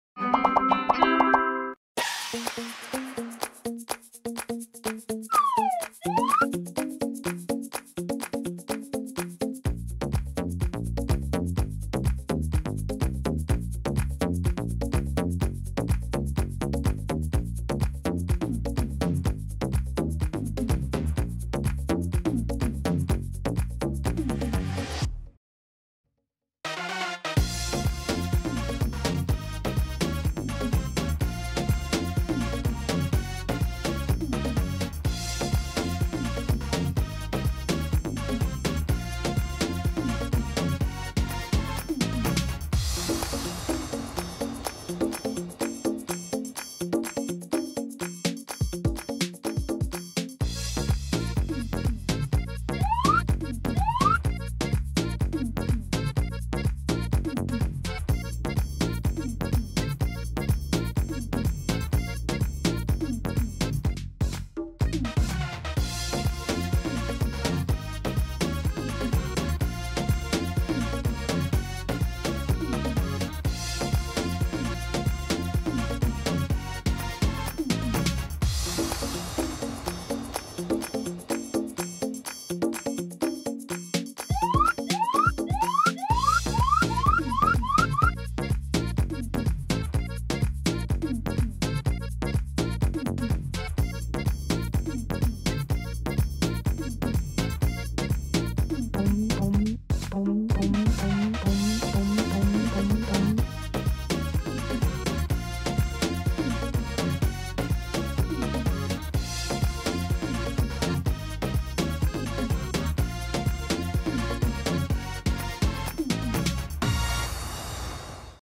Christmas tree made of plasticine How to mold figures - modeling from plasticine
How to make a Christmas tree with your own hands from plasticine